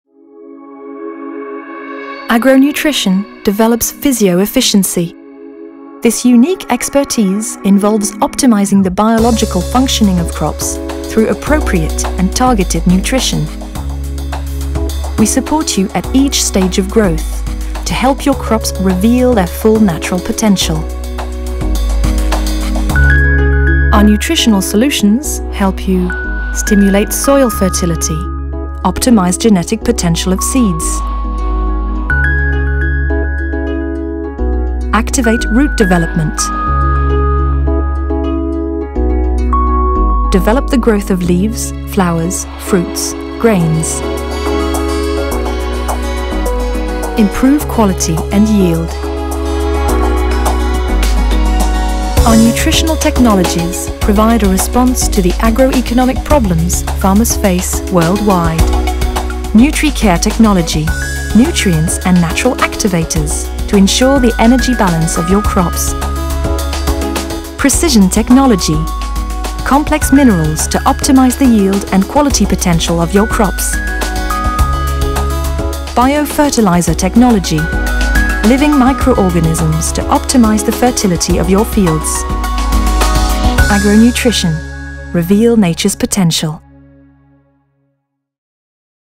Agronutrition - Our expertise : physio-efficience
Agronutrition develops physio-efficiency.

This unique expertise involves optimising the biological functioning of crops through appropriate and targeted nutrition.

We support you at each stage of growth, to help your crops reveal their full natural potential.